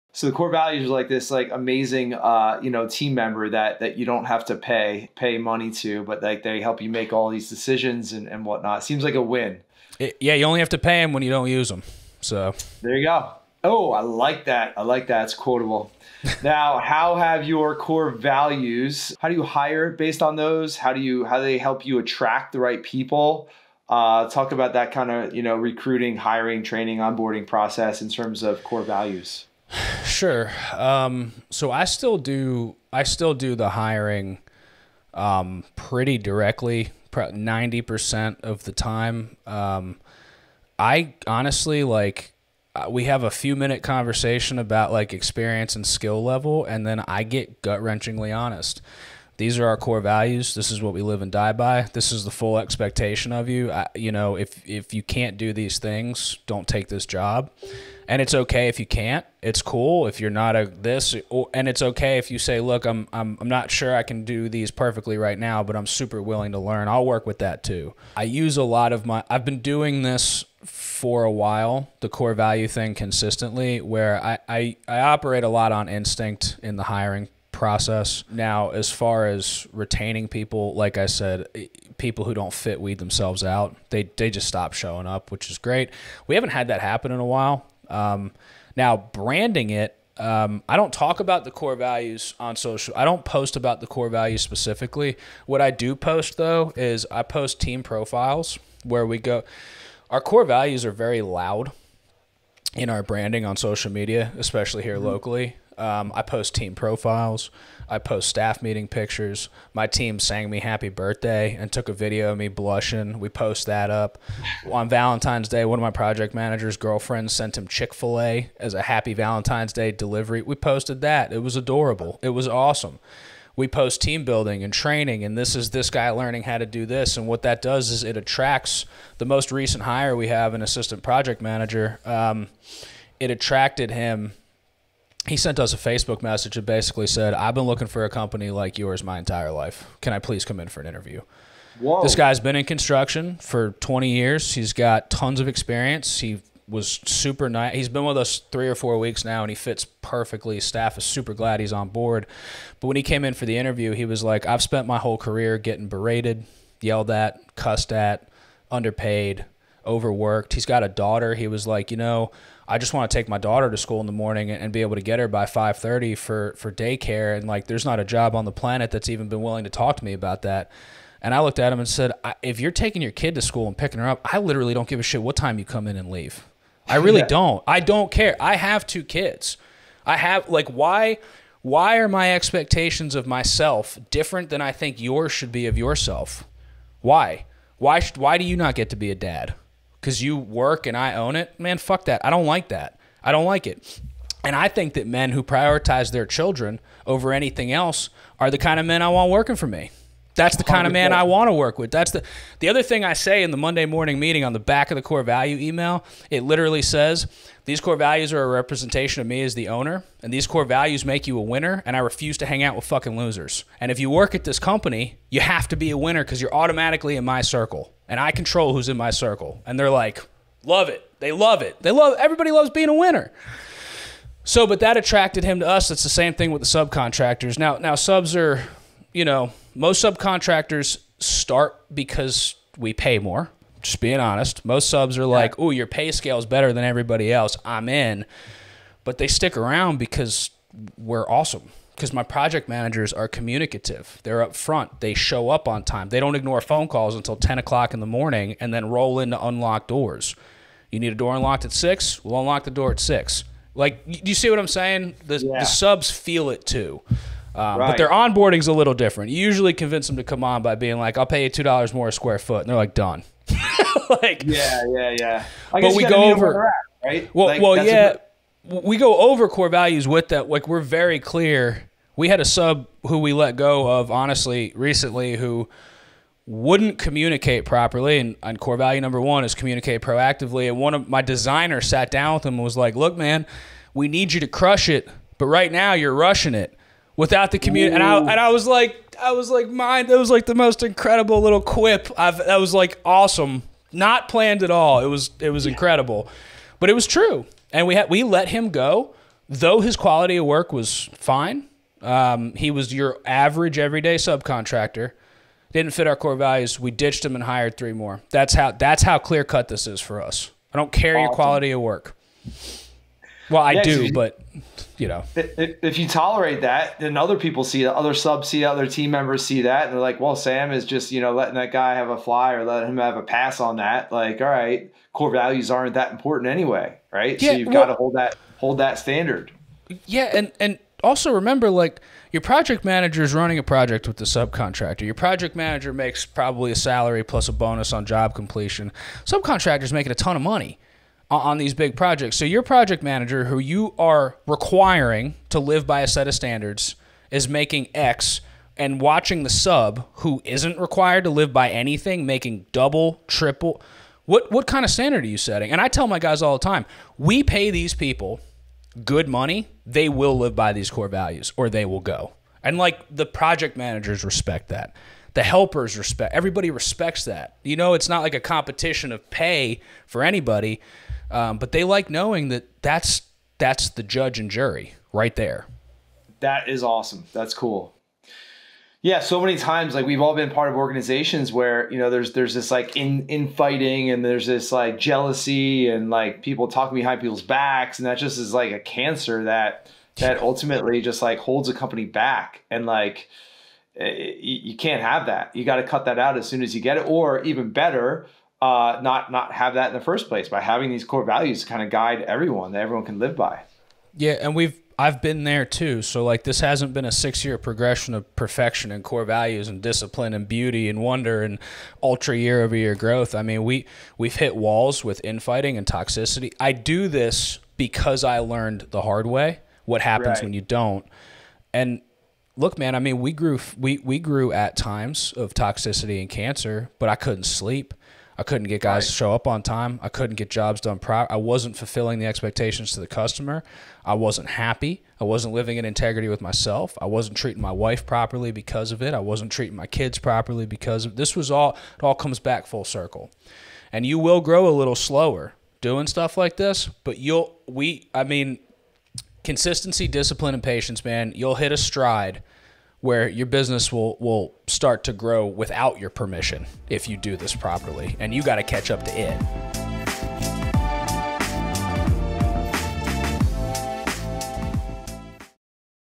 How To Attract A-Players To Your Company So Everyone WINS | Construction Growth Strategies
How To Attract A-Players To Your Company So Everyone WINS

Good companies are always on the look out for their next hires. We see too many companies putting half-measured efforts into their hiring efforts and getting frustrated because they're not able to find and retain good people.

Ask yourself - are you doing the work you need to do to attract and retain the best people?

Are you expecting amazing results without putting in the work?

Are you building a company that awesome people will be attracted to???

You need to take more OWNERSHIP.

Joseph is grateful to have had the opportunity to sit down with Sam Kaufman to hear how he has built a company and culture on Core Values, and how he has been able to build a successful company and many successful leaders in a relatively short period of time.